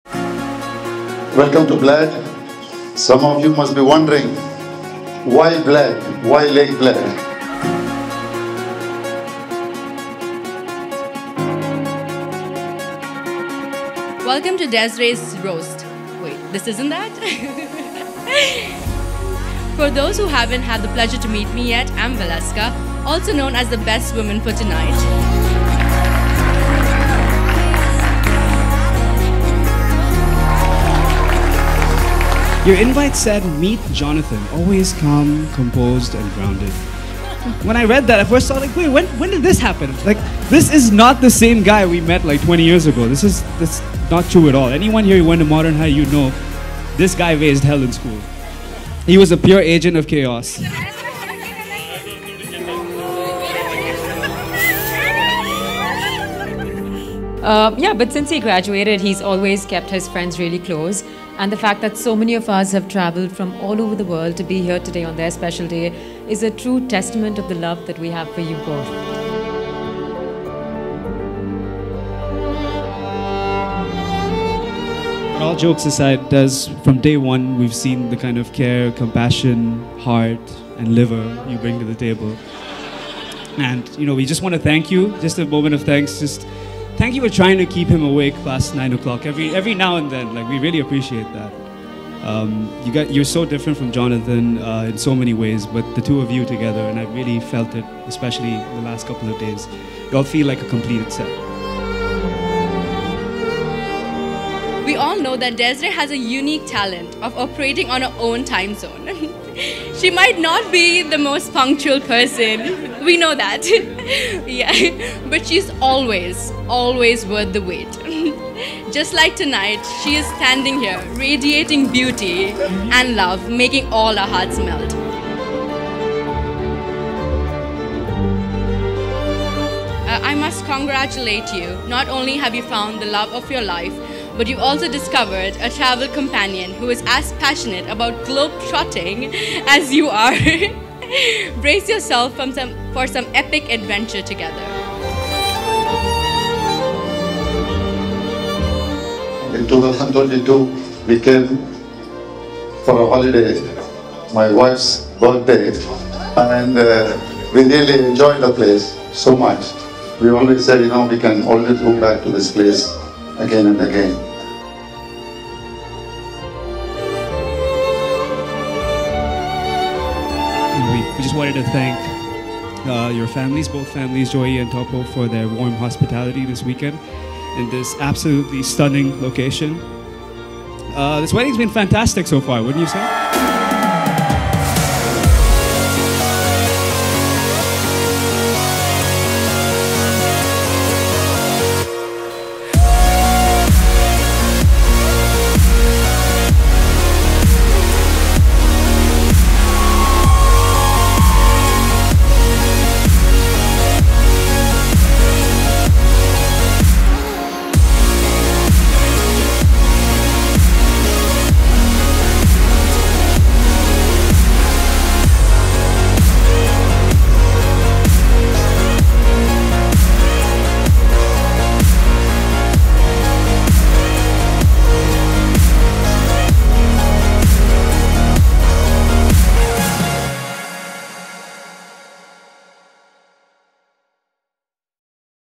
A Love Story Unveiled / Desiree & Jonathan / Lake Bled Slovenia
Dive into the magic of love at Lake Bled! 🎥✨ Our lenses captured every heartbeat, every dance move, and every joyous moment of this two-day Indian wedding extravaganza. From the vibrant traditions on day one to the sun breaking through the storm clouds on day two, it's a visual feast of love and celebration.